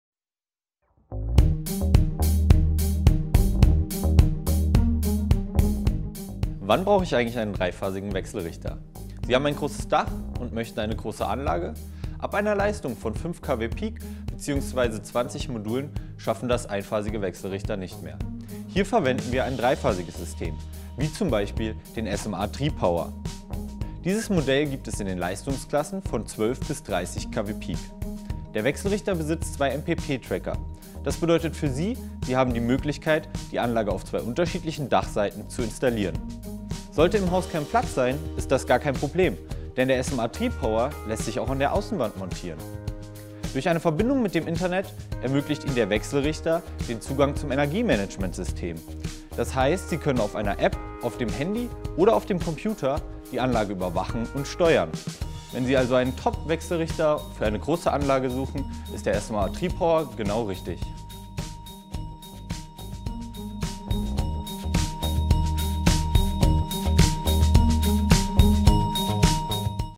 SMA Tripower – Dreiphasige Wechselrichter für große Solaranlagen
Wann braucht man einen dreiphasigen Wechselrichter?
Sie haben ein großes Dach mit einer großen Solaranlage über 5 kWp, dann schaffen das einphasige Wechselrichter nicht mehr.

Mehr zum Wechselrichter SMA Sunny Tripower 5000TL – 12000TL:

Die Tripower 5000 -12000-Modelle bezeichnen Hochleistungswechselrichter des deutschen Herstellers SMA Solar Technology, die sich vor allem durch ihre Dreiphasigkeit, aber auch seinen breiten Anwendungsbereich auszeichnen.

So wird diese Modellreihe mit einem breiten Spektrum von 5 bis 12 kW vertrieben und ist somit nicht nur für die klassische Hausdachanlage, sondern auch für den höheren Leistungsbereich geeignet. Aufgrund der bewährten Optiflex-Technologie und des asymmetrischen Multistrings sorgen diese PV-Wechselrichter mit einem Spitzenwirkungsgrad und dem Verschattungsmanagement OptiTrac Global Peak für besonders hohe Erträge. Neben der Bluetooth-Kommunikation sind selbstverständlich auch vielfältige Netzmanagementfunktionen per Internet verfügbar, die den Kunden das Gerät warten und kontrollieren lassen und die rechtlichen Anforderungen erfüllen. Aufgrund ihrer Beschaffenheit eignen sich diese Wechselrichter daher hervorragend für die Installation von privaten Aufdachanlagen, aber ebenso für größere Aufdachanlagen in den Leistungsklassen von 5 bis 12 kW.

Vorteile des SMA Tripower:

- Hohe Wirtschaftlichkeit
- Erhöhte Flexibilität durch Einsatz eines Geräts bei großer Anlage
- sehr leise mit 40 dB
- Monitoring/Überwachung per App und PC
- Integration mit SMA-basierten Speichersystemen